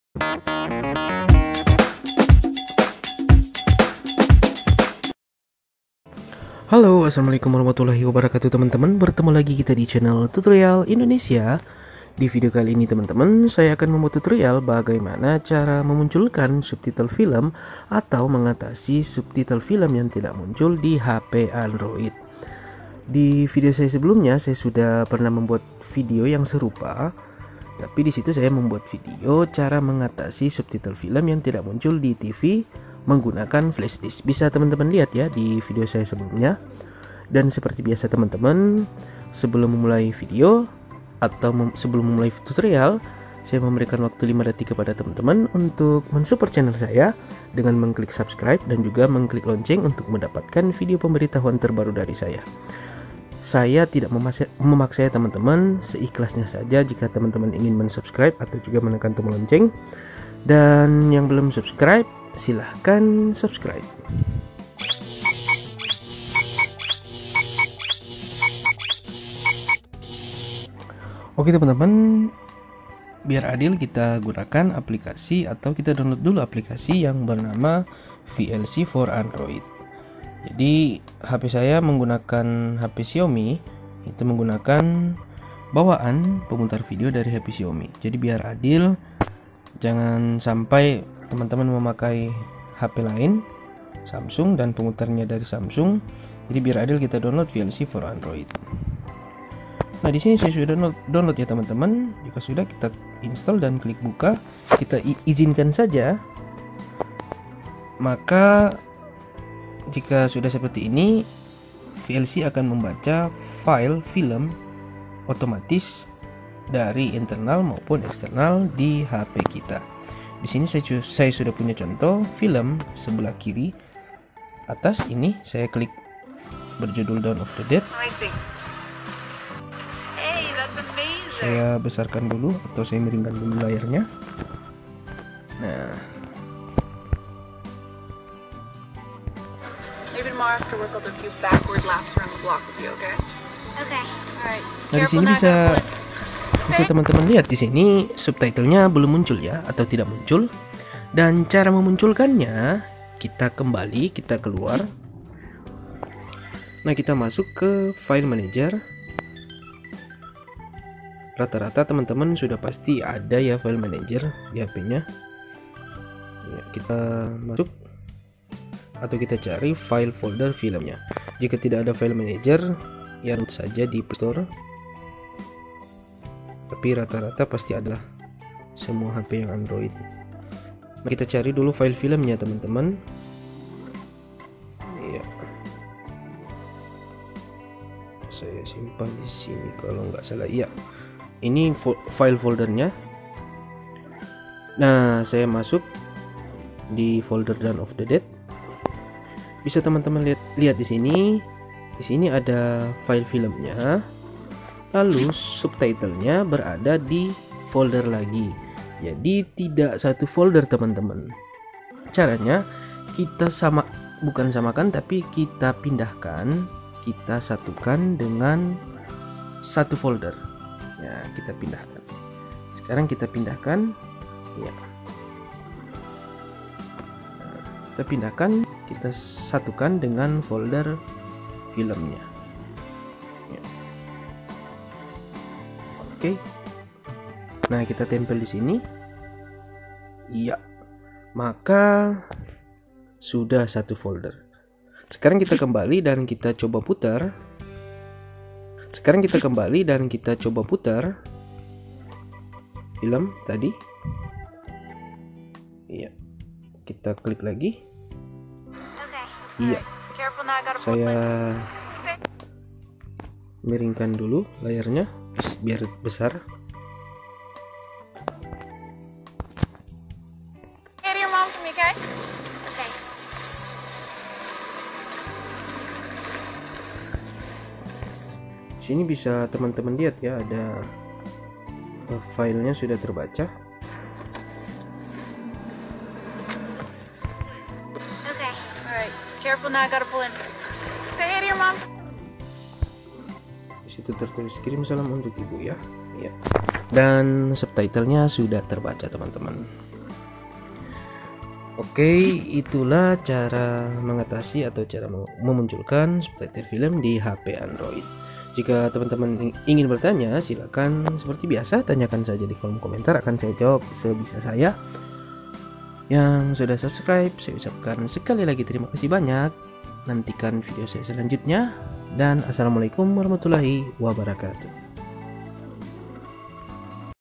Cara Mengatasi Subtitle film tidak muncul di hp android
assalamualaikum warahmatullahi wabarakatuh teman2 bertemu lagi dengan tutorial Indonesia di video kali ini saya kembali membuat tutorial,cara mengatasi subtitle film tidak muncul di semua hp android..mau hp apa pun insyaallah pasti berhasil..
Siapa tau ada yang mau berbagi sedikit rejeki buat saya
Sekecil apapun sangat berarti buat saya,terima kasih sebelumnya :)